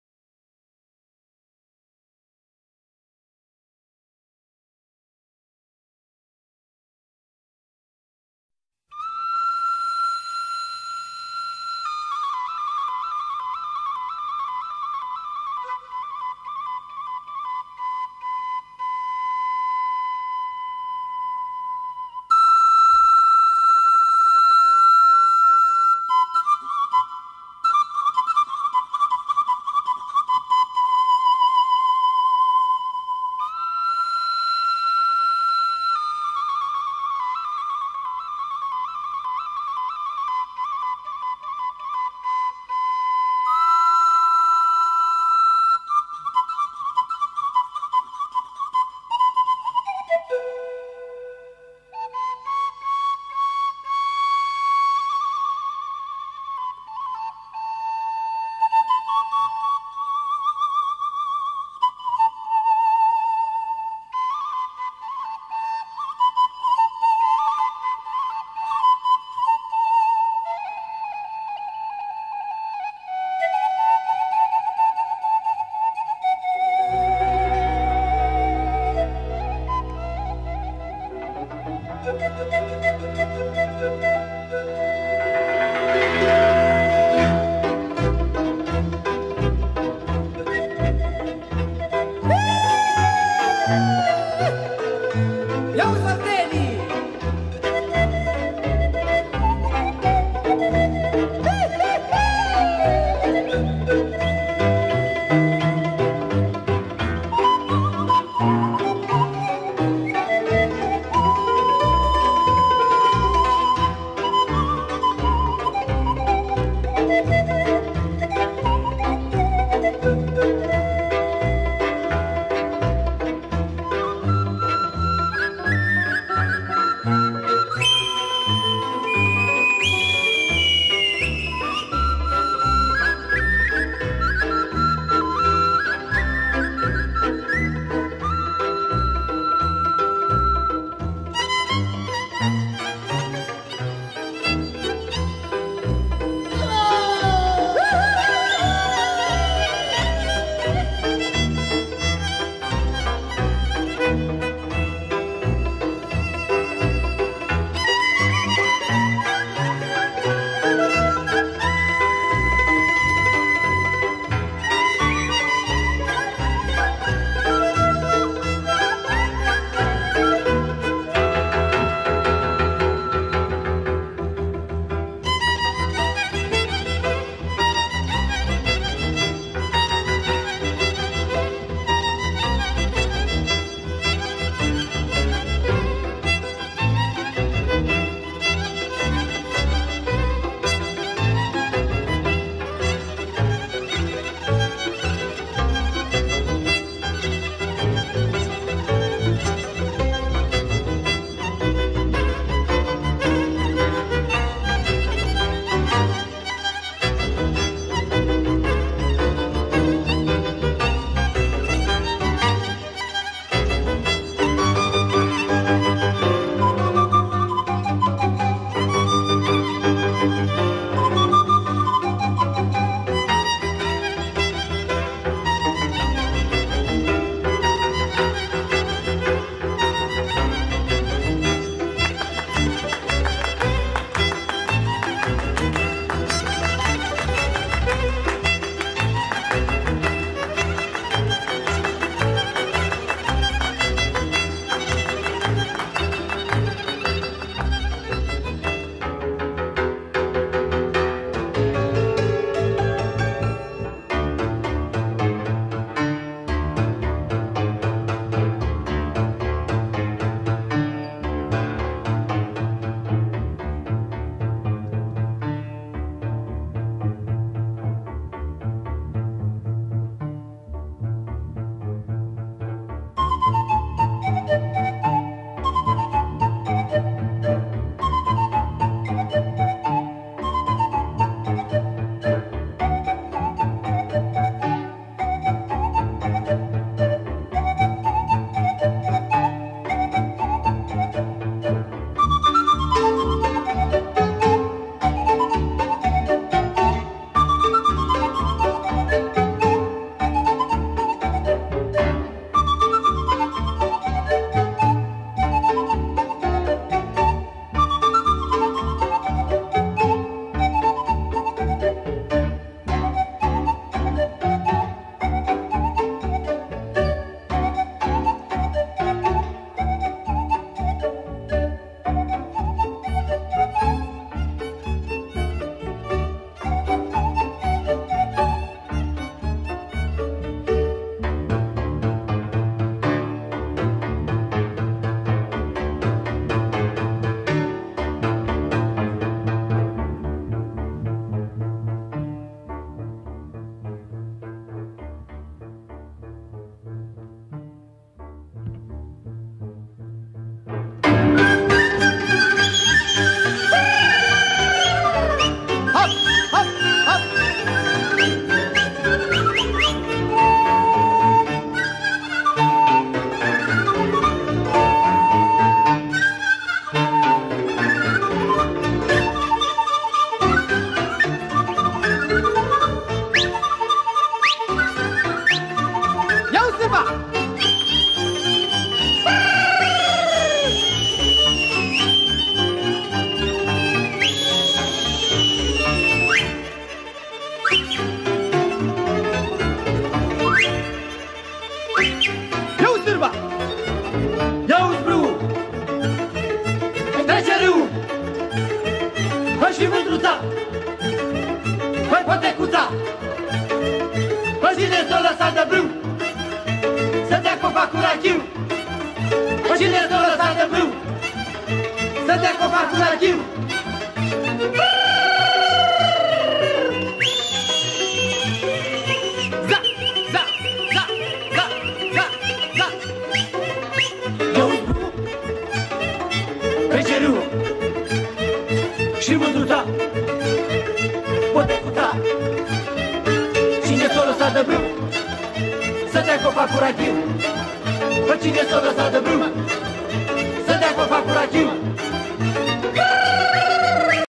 Mysterious Romania
A promotional movie made by ADR-Tourism, an ngo who voluntarilly promotes tourism to Romania.